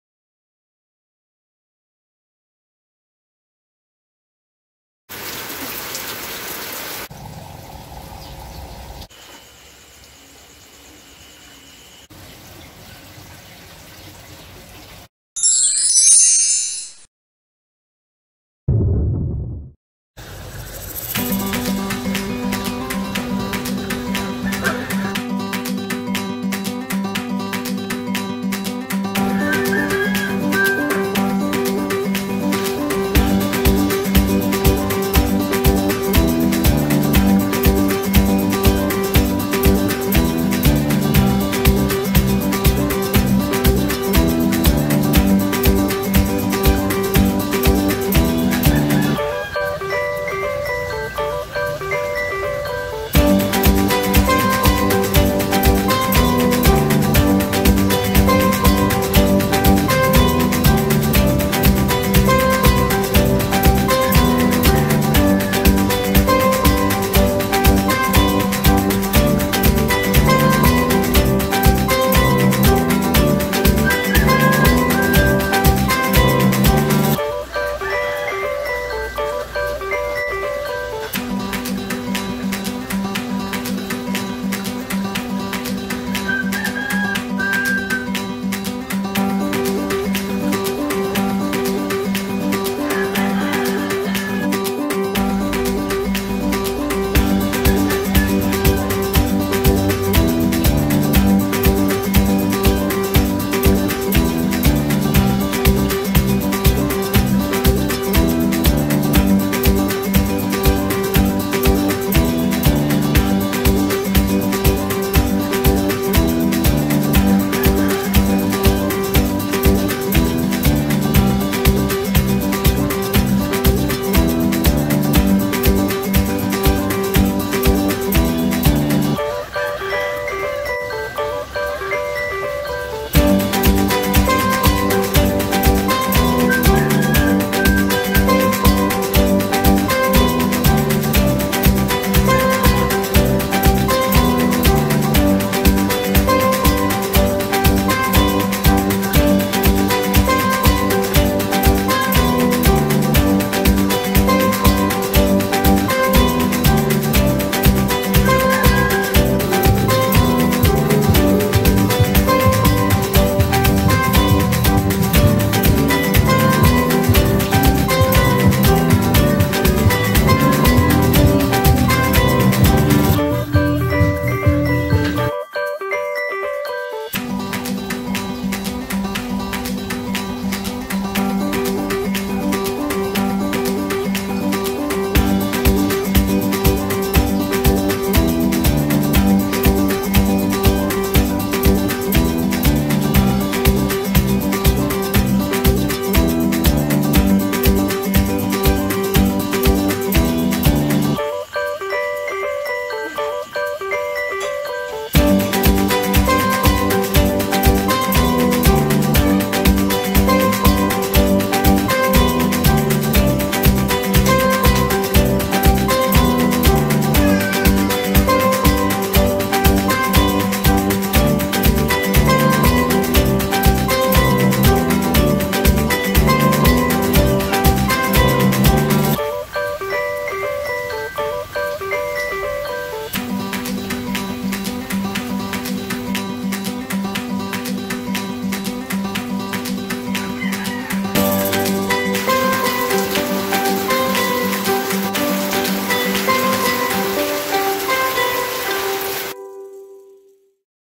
TETRA FISH 100 % BREEDING SUCCESFUL// Fish Breeding Tutorial 4
THANK YOU LORD!
I want to share with you my video on tetra fish breeding.
sorry for the long wait of uploading this video.
hope you learn.
I'm your Teacher JIE and this is my discovery.